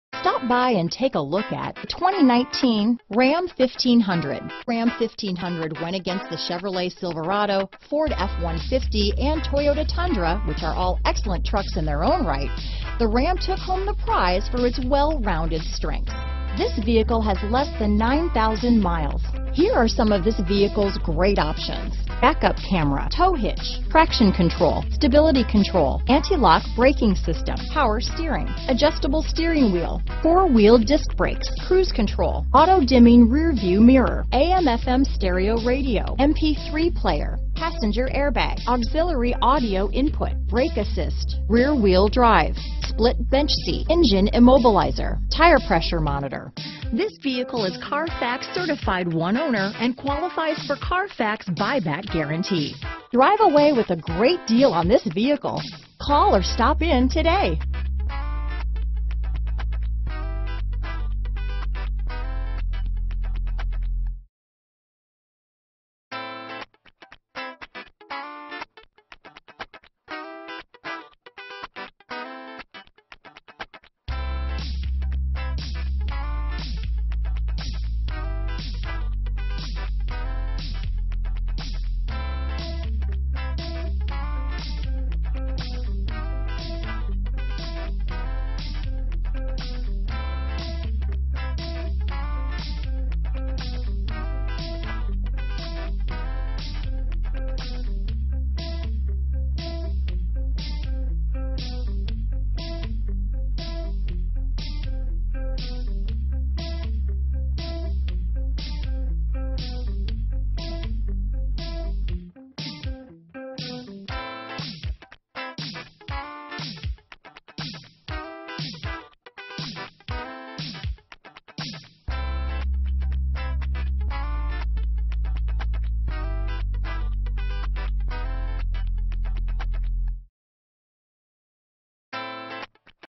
2019 Ram 1500 Classic Marion, Carbondale, Mt Vernon, Evansville, IL X9275
Bright White Clearcoat Used 2019 Ram 1500 Classic available in Marion, Illinois at Jim Hayes Inc. Servicing the Carbondale, Mt Vernon, Evansville, IL area.
2019 Ram 1500 Classic Tradesman - Stock#: X9275 - VIN#: 3C6JR6DT6KG560676

Jim Hayes
2130 US-45

Only 8,975 Miles! Delivers 22 Highway MPG and 15 City MPG! This Ram 1500 Classic delivers a Regular Unleaded V-8 5.7 L/345 engine powering this Automatic transmission. WHEELS: 17 X 7 ALUMINUM, TRANSMISSION: 8-SPEED AUTOMATIC (8HP70), TIRES: P265/70R17 BSW AS (STD).* This Ram 1500 Classic Features the Following Options *QUICK ORDER PACKAGE 27B TRADESMAN -inc: Engine: 5.7L V8 HEMI MDS VVT, Transmission: 8-Speed Automatic (8HP70), Tradesman Package , POWER & REMOTE ENTRY GROUP -inc: Exterior Mirrors w/Heating Element, Power Windows w/Front 1-Touch Down, Power Heated Fold-Away Mirrors, Black Exterior Mirrors, Premium Vinyl Door Trim w/Map Pocket, Power Locks, Remote Keyless Entry w/All-Secure, MANUFACTURER'S STATEMENT OF ORIGIN, ENGINE: 5.7L V8 HEMI MDS VVT, DIESEL GRAY/BLACK, CLOTH 40/20/40 BENCH SEAT, DELETE SPRAY IN BEDLINER, BRIGHT WHITE CLEARCOAT, BLACK SEATS, 3.21 REAR AXLE RATIO (STD), Variable Intermittent Wipers.* Stop By Today *Test drive this must-see, must-drive, must-own beauty today at Jim Hayes, Inc., 2130 Us 45 North, Harrisburg, IL 62946.